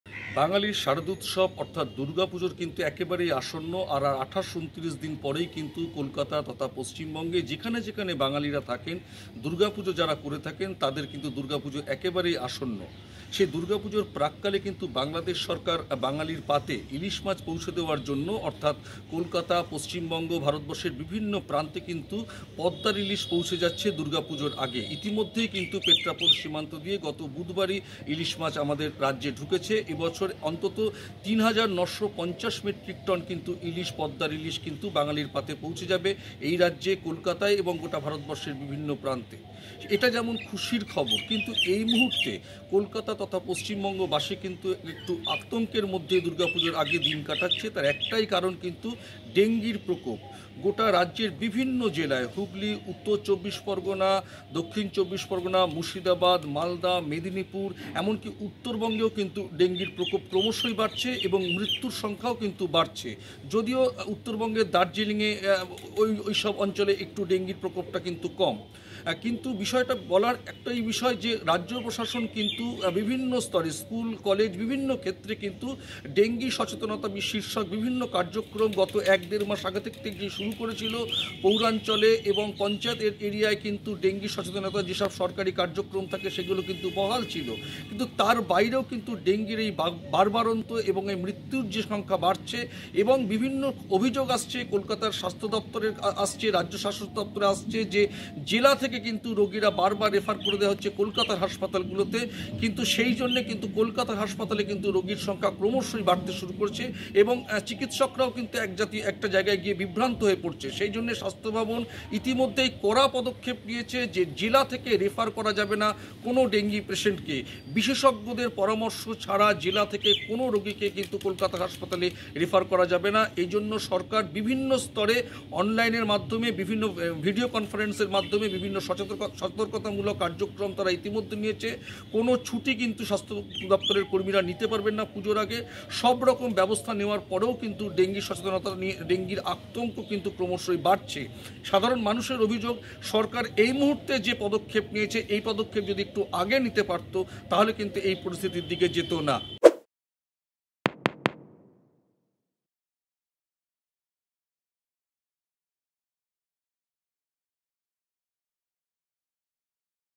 দুর্গা পূজার আগে কলকাতাসহ পুরো পশ্চিমবঙ্গে বাড়ছে ডেঙ্গুর প্রকোপ | Dangu | Rtv News
২৪ ঘণ্টায় ডেঙ্গুতে ২১ জনের মৃ'ত্যু, নতুন আক্রান্ত তিন হাজার।